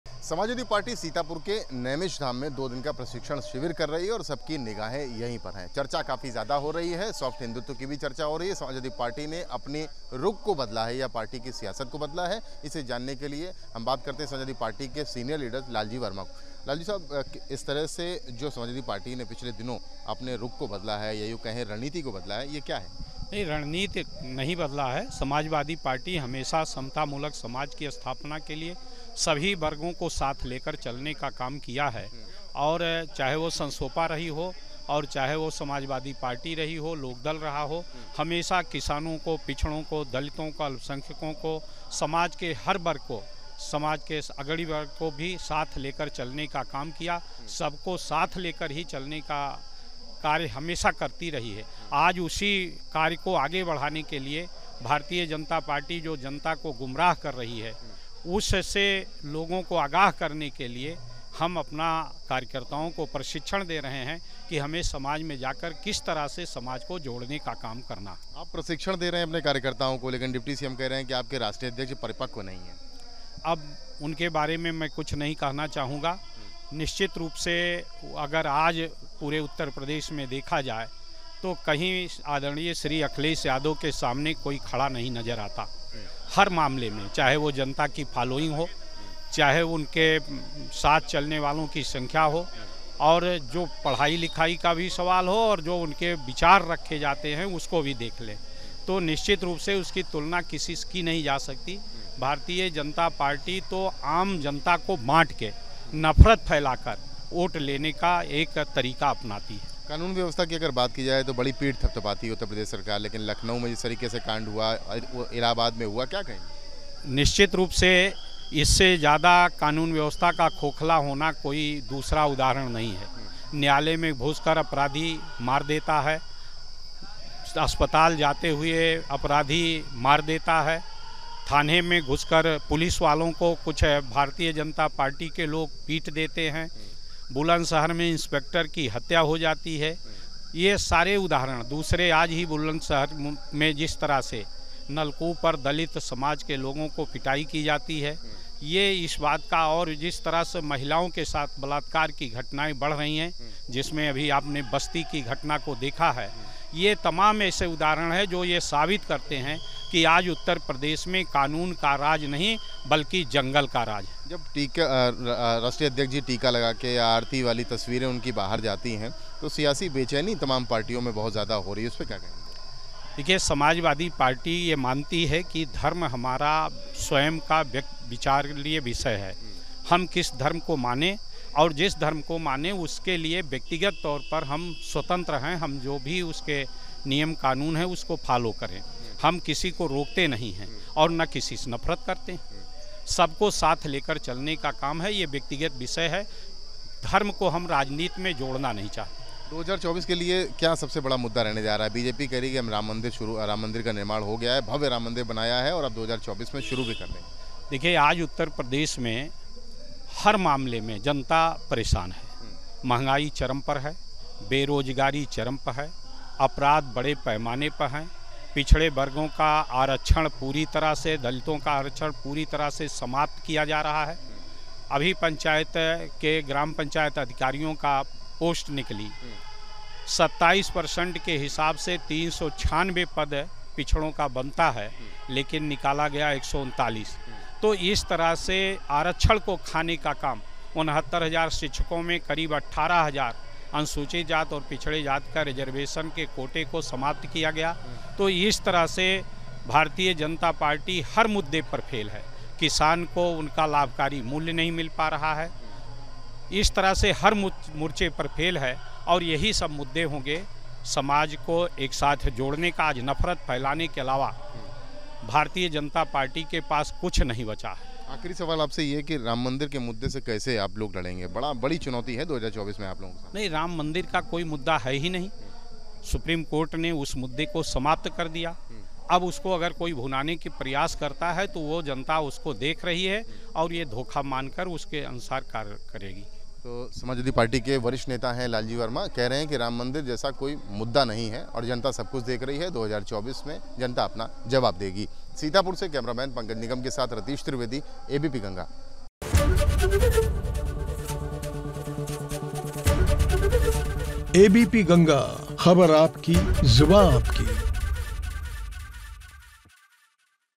सपा नेता Lalji Verma ने Akhilesh Yadav की तारीफ में क्या कह दिया ?
समाजवादी पार्टी के राष्ट्रीय महासचिव लालजी वर्मा भी प्रषिक्षण शिविर में पहुंचे हैं। उनका कहना है कि पार्टी अपने बूथ अध्यक्ष और सेक्टर प्रभारियों को मांझने का काम कर रही है। साल 2024 में कार्यकर्ताओं को बीजेपी से कैसे निपटना होगा इसकी भी चर्चा हो रही है। समता मूलक समाज की स्थापना का काम सपा ने हमेशा किया है। आज उसी काम को आगे बढ़ाने और बीजेपी की गुमराह करने वाली नीति से बचाने का काम करेंगे। डिप्टी सीएम ने अखिलेश जी पर जो कुछ कहा उसपर कुछ नहीं कहूंगा। अखिलेश यादव के सामने कोई नहीं खड़ा होता। बीजेपी जनता को बांटकर, नफरत फैलाकर राजनीति करती है। यूपी में कानून व्यवस्था खोखली हो चुकी है। बुलंदशहर में दलितों की पिटाई हो रही है। सियासी बेचैनी पर कहा कि धर्म स्वयं का विचार है, व्यक्तिगत तौर पर किया जाने वाला विषय है धर्म को राजनीति से हम लोग नहीं जोड़ते। यूपी में जनता महंगाई, बेरोजगारी से परेशान है और आरक्षण खत्म किया जा रहा है। राम मंदिर कोई मुद्दा ही नहीं है लोकसभा चुनावों के लिए। उनसे बात की हमारे संवाददाता रतीश त्रिवेदी ने।